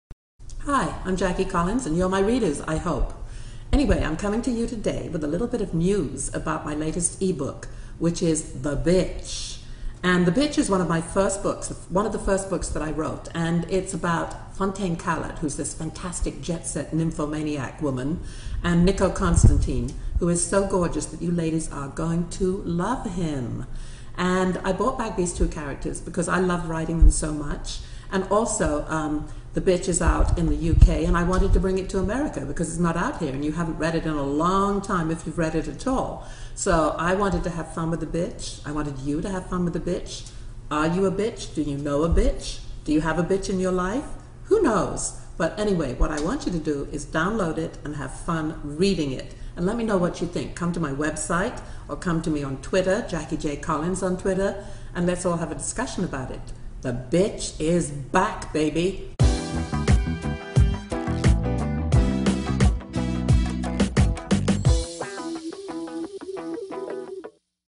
Jackie Collins Introduces Her Book THE BITCH
Jackie's announcement of her new eBook - coming to the U.S. for the first time April 17.